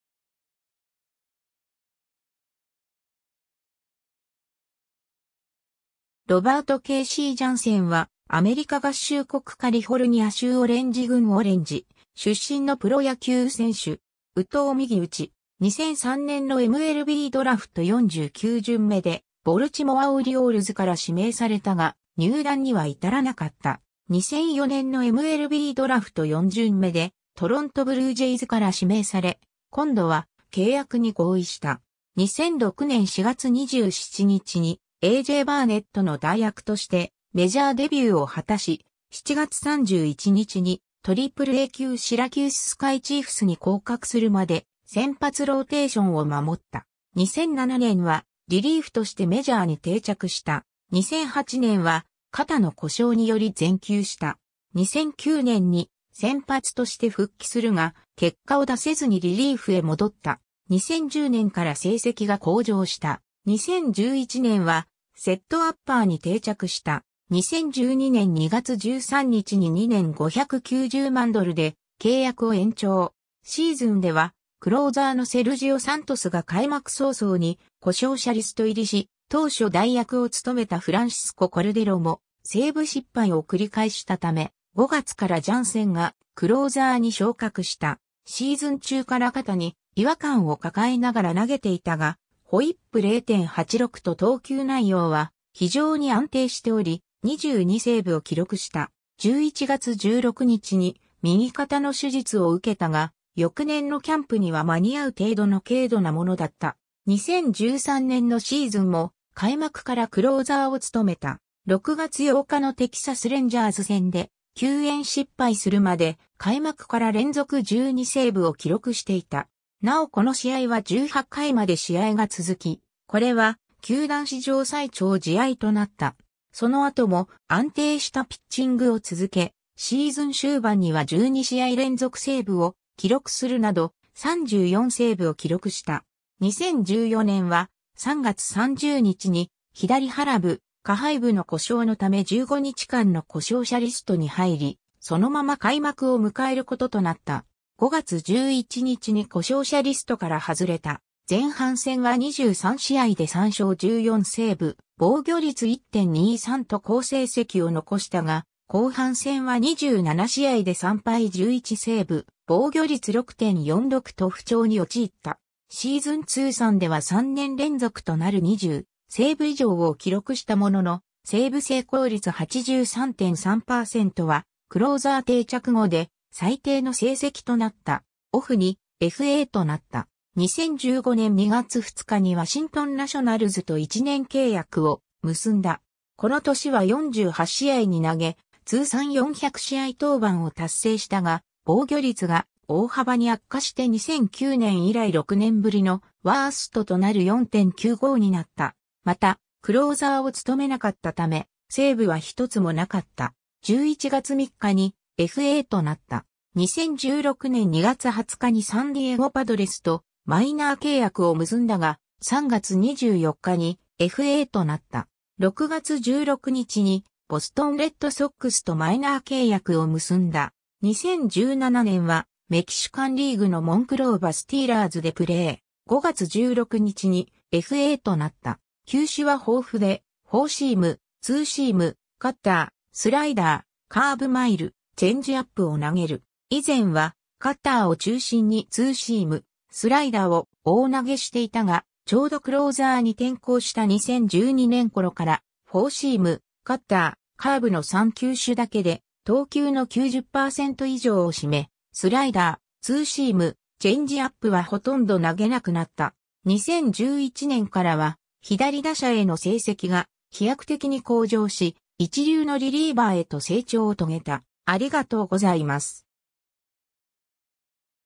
ケイシー・ジャンセン
モンクローバ・スティーラーズの選手
ロバート・ケイシー・ジャンセン（Robert Casey Janssen, 1981年9月17日 - ）は、アメリカ合衆国カリフォルニア州オレンジ郡オレンジ出身のプロ野球選手（投手）。
右投右打。
 2003年のMLBドラフト49巡目（全体1435位）でボルチモア・オリオールズから指名されたが、入団には至らなかった。
 2004年のMLBドラフト4巡目（全体117位）でトロント・ブルージェイズから指名され、今度は契約に合意した。
 2006年4月27日にA.J.バーネットの代役としてメジャーデビューを果たし、7月31日にAAA級シラキュース・スカイチーフスに降格するまで先発ローテーションを守った。
 2007年は、リリーフとしてメジャーに定着した。
 2008年は、肩の故障により全休した。
 2009年に、先発として復帰するが、結果を出せずにリリーフへ戻った。
 2010年から成績が向上した。
 2011年は、セットアッパーに定着した。
 2012年2月13日に2年590万ドル（3年目は400万ドルのオプション）で契約を延長。
シーズンではクローザーのセルジオ・サントスが開幕早々に故障者リスト入りし、当初代役を務めたフランシスコ・コルデロもセーブ失敗を繰り返したため、5月からジャンセンがクローザーに昇格した。
シーズン中から肩に違和感を抱えながら投げていたが、WHIP0.86と投球内容は非常に安定しており、22セーブを記録した。
11月16日に右肩の手術を受けたが、翌年のキャンプには間に合う程度の軽度なものだった。
 2013年のシーズンも、開幕からクローザーを務めた。
6月8日のテキサス・レンジャーズ戦で救援失敗するまで、開幕から連続12セーブを記録していた。
なおこの試合は18回まで試合が続き、これは球団史上最長試合となった（試合はブルージェイズが勝利）。
その後も、安定したピッチングを続け、シーズン終盤には12試合連続セーブを記録するなど、34セーブを記録した。
 2014年は、3月30日に左腹部・下背部の故障のため15日間の故障者リストに入り、そのまま開幕を迎えることとなった。
5月11日に故障者リストから外れた。
前半戦は23試合で3勝14セーブ・防御率1.23と好成績を残したが、後半戦は27試合で3敗11セーブ・防御率6.46と不調に陥った。
シーズン通算では3年連続となる20セーブ以上を記録したものの、セーブ成功率83.3%はクローザー定着後で最低の成績となった。
 2015年2月2日にワシントン・ナショナルズと1年契約を結んだ。
この年は48試合に投げ、通算400試合登板を達成したが、防御率が大幅に悪化して2009年以来6年ぶりのワーストとなる4.95になった。
また、クローザーを務めなかった為、セーブは1つもなかった。
11月3日にFAとなった。
 2016年2月20日にサンディエゴ・パドレスとマイナー契約を結んだが、3月24日にFAとなった。
6月16日にボストン・レッドソックスとマイナー契約を結んだ。
 2017年はメキシカンリーグのモンクローバ・スティーラーズでプレー。
5月16日にFAとなった。
 球種は豊富で、フォーシーム（90～94マイル）、ツーシーム（90～94マイル）、カッター（88～92マイル）、スライダー（83～87マイル）、カーブ（74～78）マイル、チェンジアップ（85～88マイル）を投げる。
 以前は、カッターを中心にツーシーム、スライダーを多投していたが、ちょうどクローザーに転向した2012年頃から、フォーシーム、カッター、カーブの3球種だけで投球の90%以上を占め、スライダー、ツーシーム、チェンジアップはほとんど投げなくなった。
2011年からは左打者への成績が飛躍的に向上し、一流のリリーバーへと成長を遂げた。